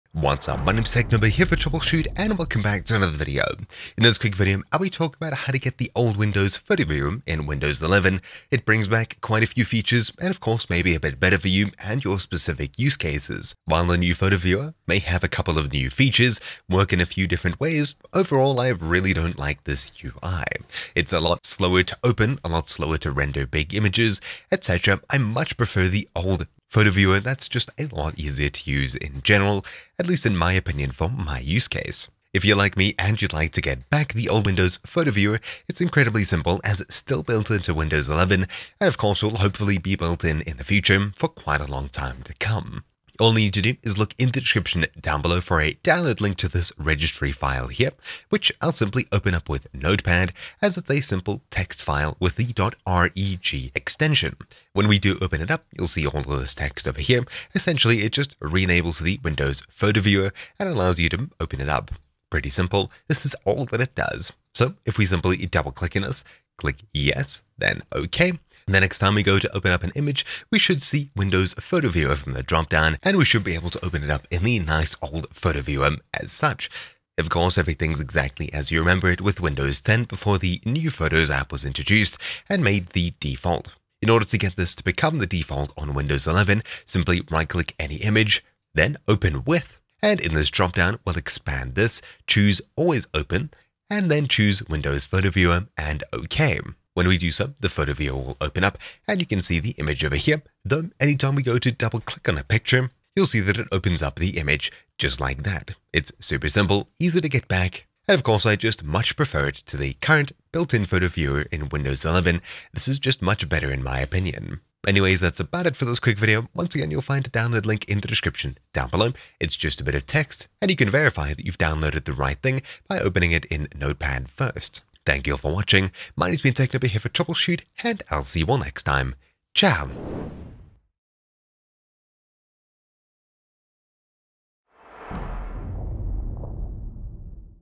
Old Photo Viewer in Windows 11 | Restore/Bring Back Windows 10 Viewer
Want to bring back the original Windows Photo Viewer in Windows 11? Well, this video shows you everything you need to know about bringing it back. With a simple Registry script, you can reenable what's already built into but hidden in the latest editions of Windows 11.

Timestamps:
0:00 - Explanation
0:42 - What does the script do?
1:04 - Enable Windows photo Viewer
1:23 - Set old Photo Viewer as Default in Windows 11